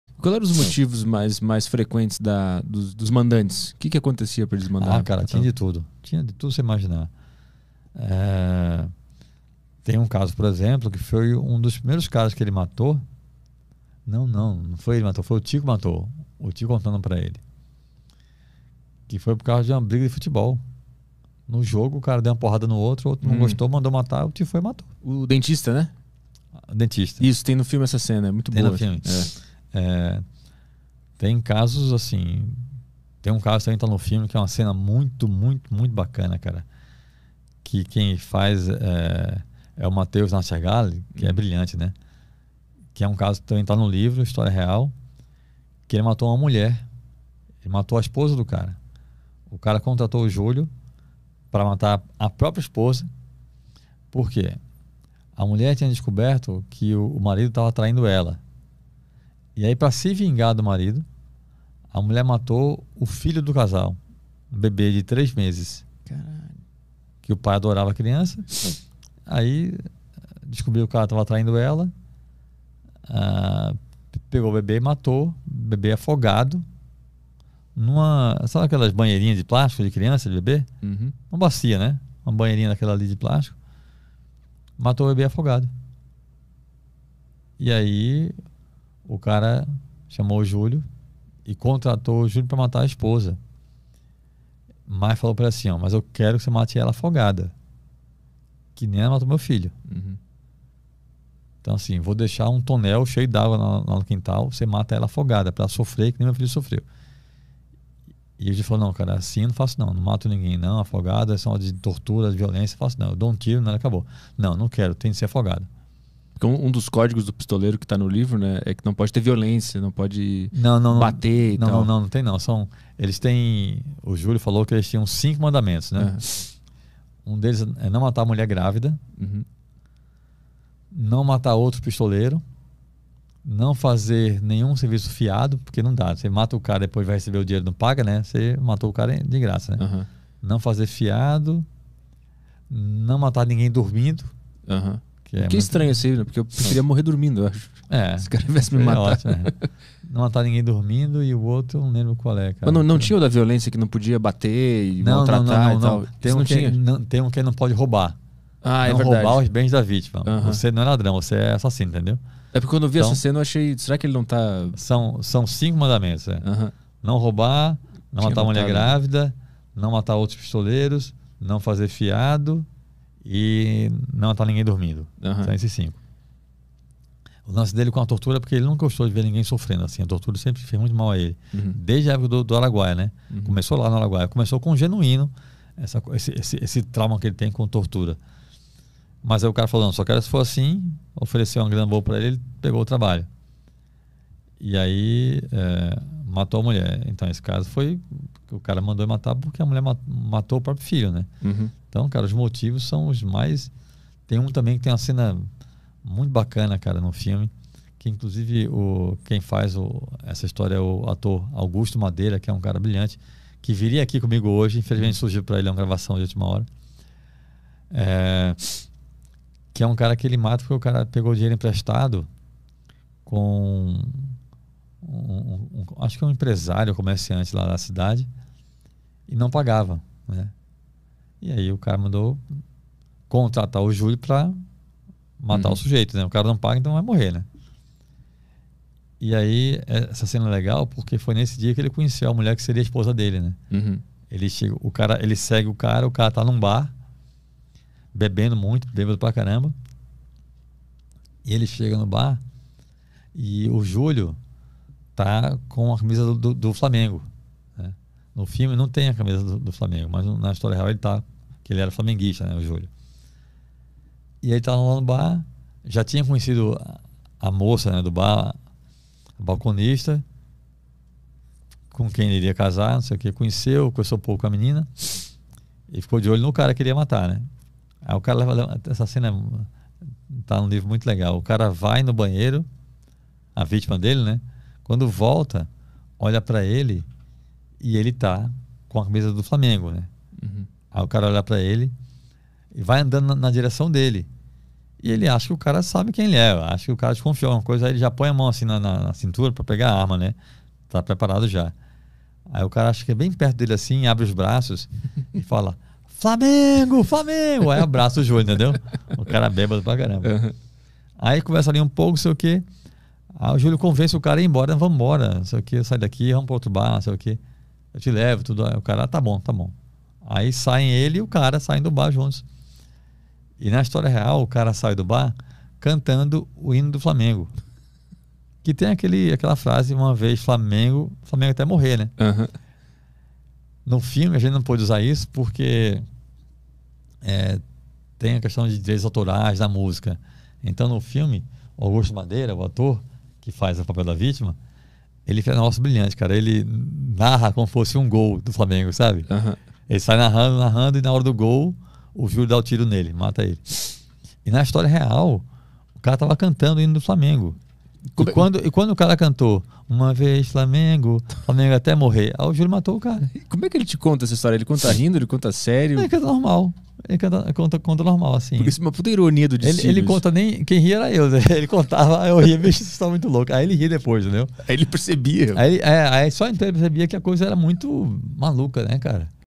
OS MOTIVOS QUE OS MANDANTES CONTRATAVAM O PISTOLEIRO | PT CLIPS
KLESTER FALA SOBRE OS MOTIVOS DE MUITOS CONTRATAREM O JULIO SANTANA COMO PISTOLEIRO E DE MUITAS DAS VEZES ELE SE NEGAR PELA "ÉTICA" DE SEU TRABALHO